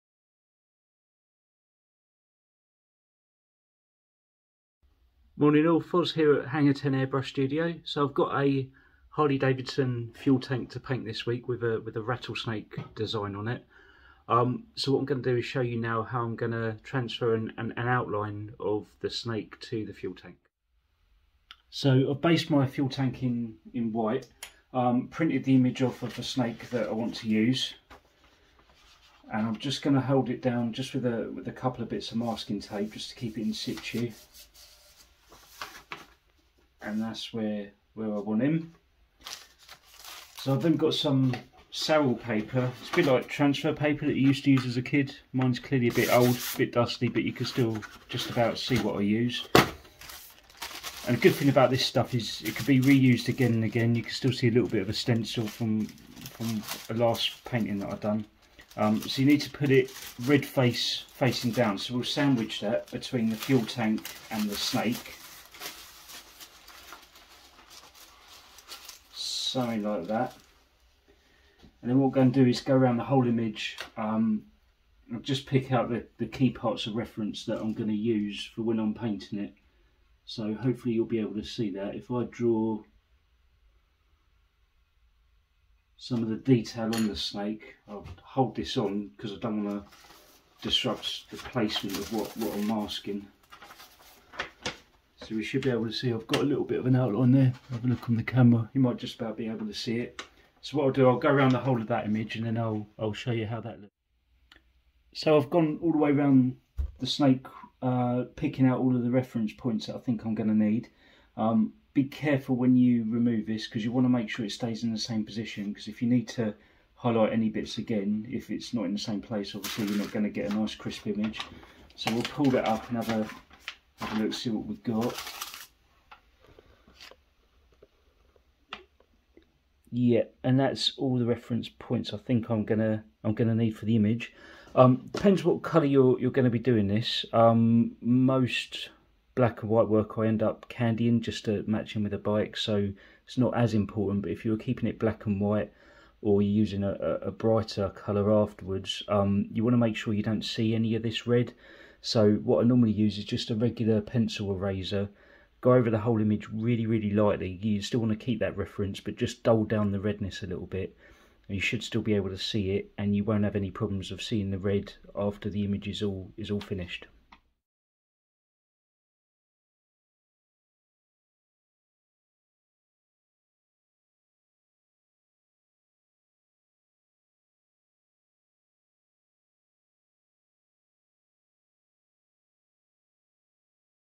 How to transfer reference outline to surface for airbrushing project - technique tutorial
Also be sure to check out the links below for more content!